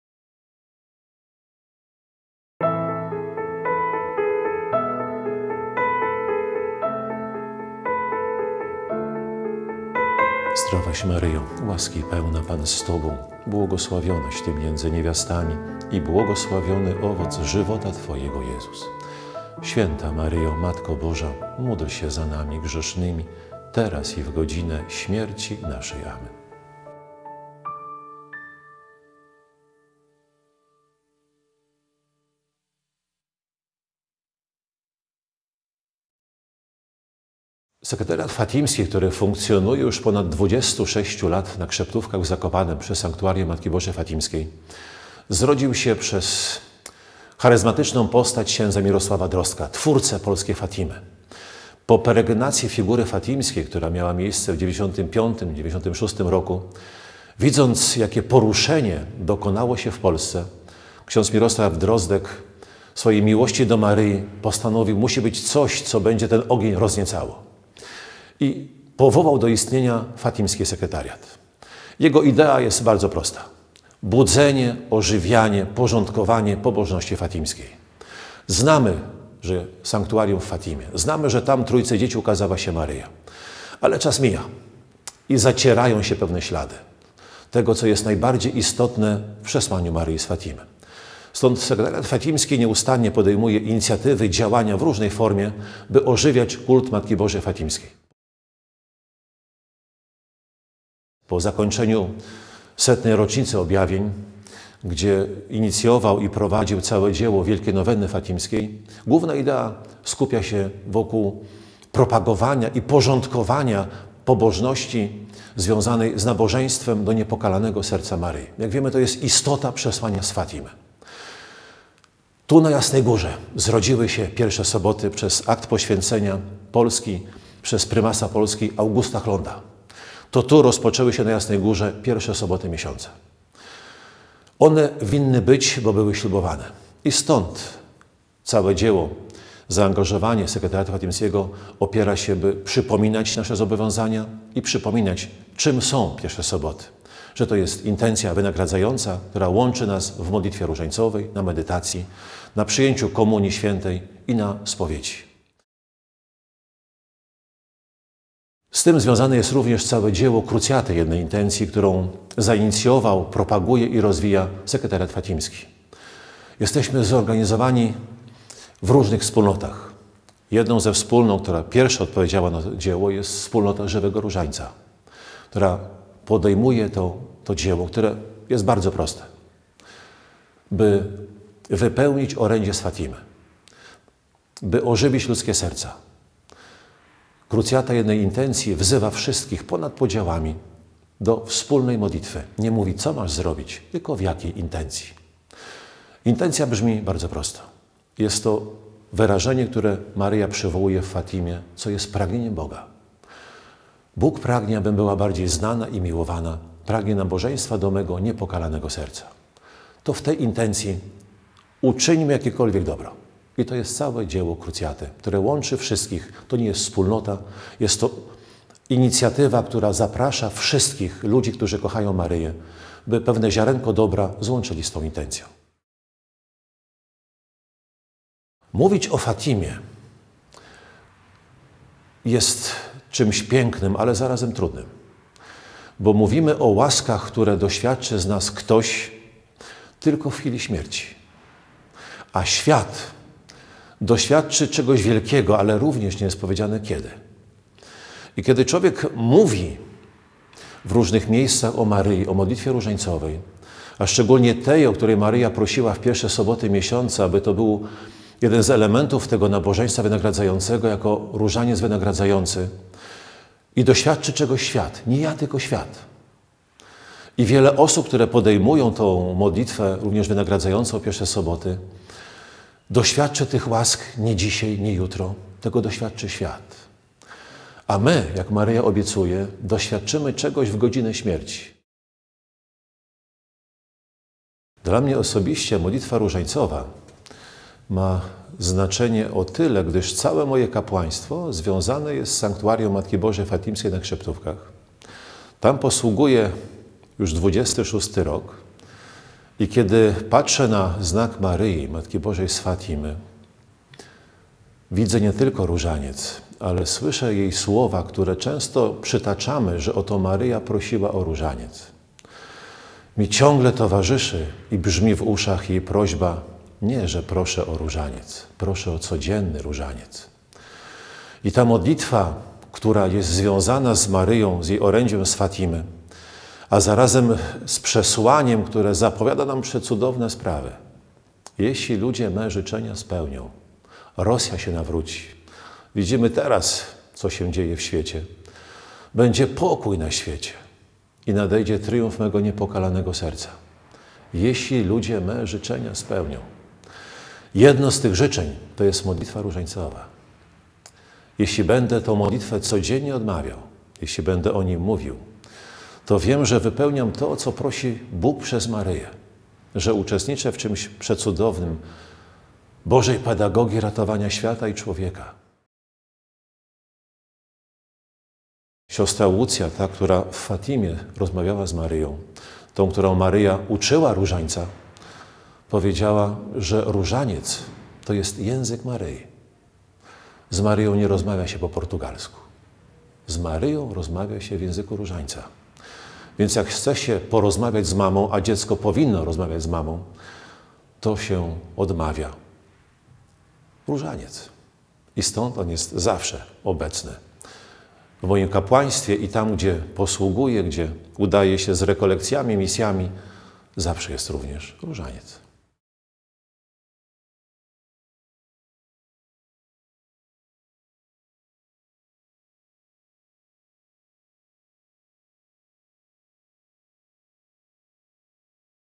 Świadectwo różańcowe – ks. Krzysztof Czapla
Tygodnik Katolicki „Niedziela” prezentuje telewizyjny cykl pt. „Uczynić Różaniec modlitwą wszystkich”, w którym przedstawimy 20 świadectw różańcowych przedstawicieli wspólnot działających w Polsce, skupiających osoby, które kochają i codziennie odmawiają Różaniec.

Uczynić Różaniec modlitwą wszystkich - to słowa i gorące pragnienie Pauliny Jaricot, założycielki Dzieła Rozkrzewiana Wiary i Żywego Różańca. Słowa te to jednocześnie hasło X Ogólnopolskiej Pielgrzymki Żywego Różańca połączonej z II Ogólnopolskim Kongresem Różańcowym, które odbędą się w dniach 3-4 czerwca br. na Jasnej Górze.